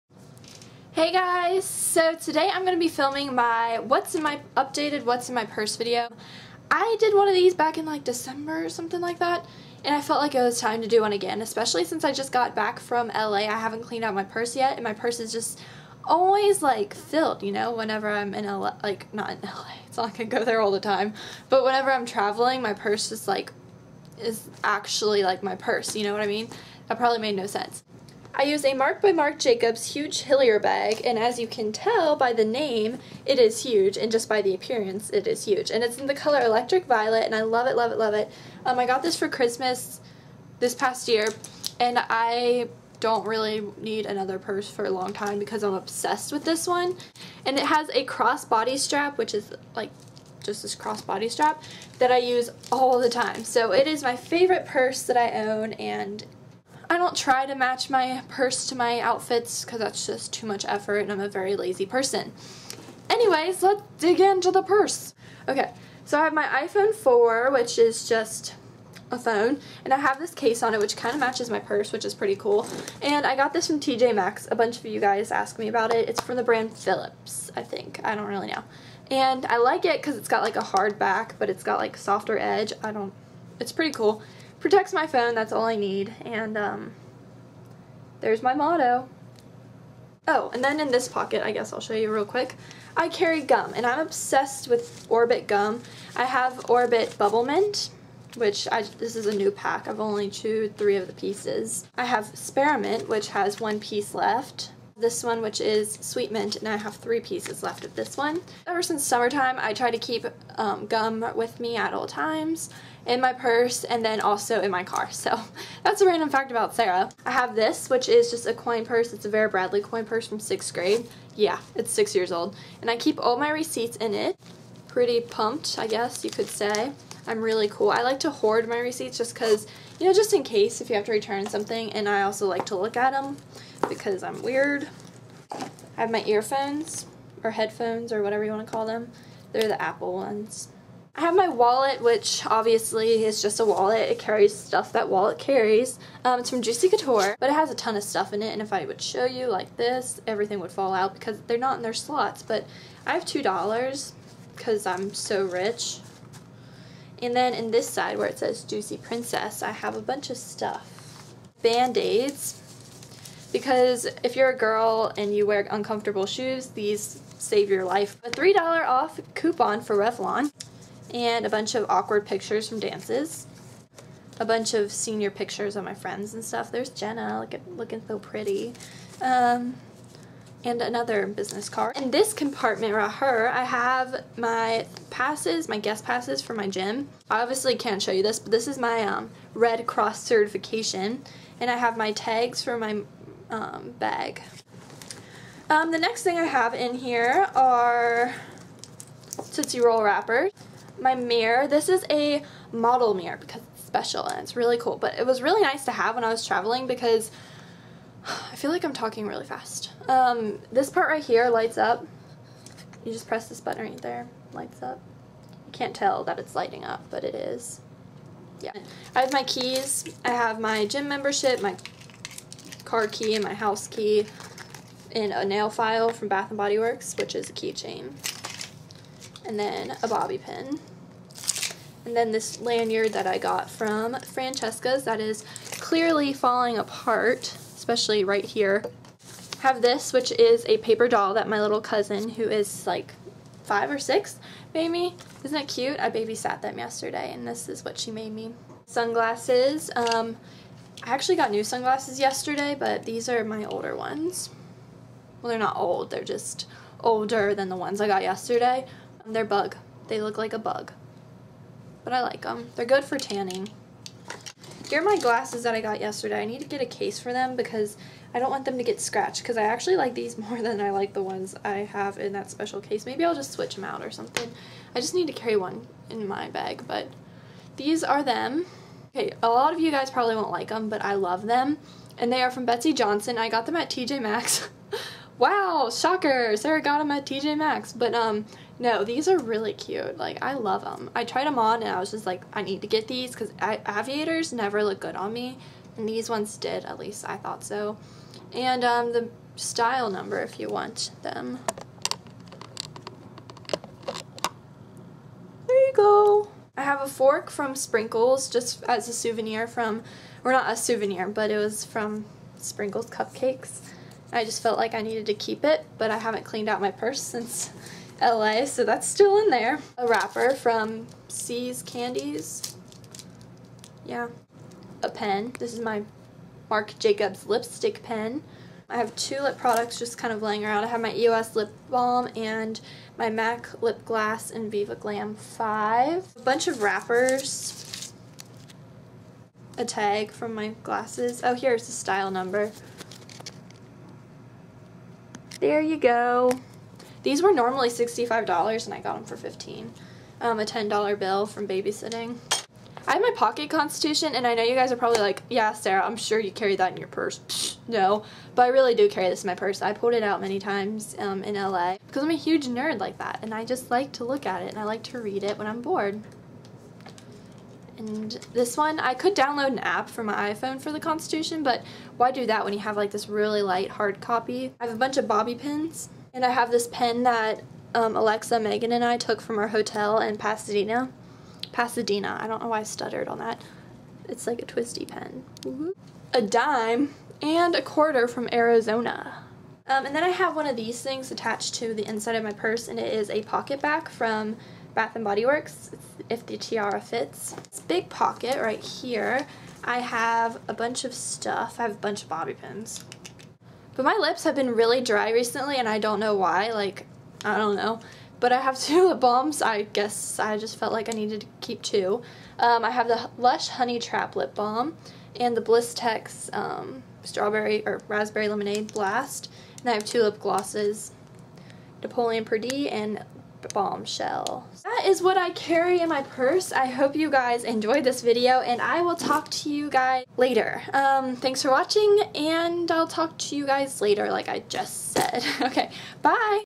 What's In My Purse? (Updated)
Just because I love watching these kinds of videos...

MY PURSE:
Marc by Marc Jacobs Huge Hillier Bag in "Electric Violet" but my color was a limited edition color so it is discontinued!

Sunglasses:
Betsey Johnson: BJ23568
Chloe: 2119

Camcorder: Panasonic TM 80

WHAT AM I WEARING:
Shirt: H&M
Earrings: Pearls
Nails: Sally Hansen Pacific Blue
Eyes: Stilia Kitten + Urban Decay Half Baked/Smog